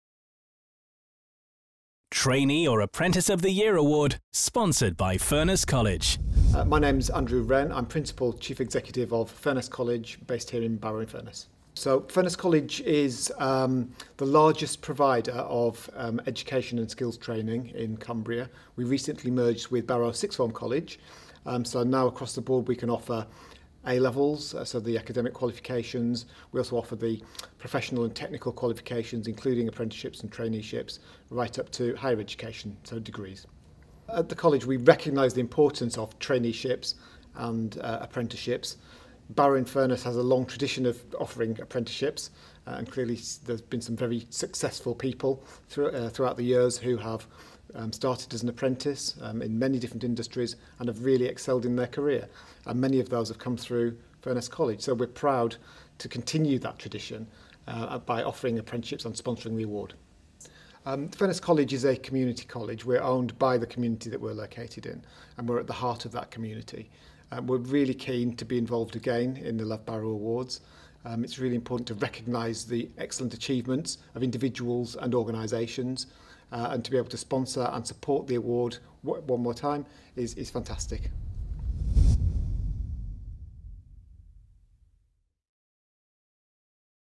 Trainee or Apprentice of the Year 2017 Sponsor - Furness College
Thanks to Furness College for kindly sponsoring the 2017 Sports Volunteer of the Year Award.
This year's winner is Bob Davies.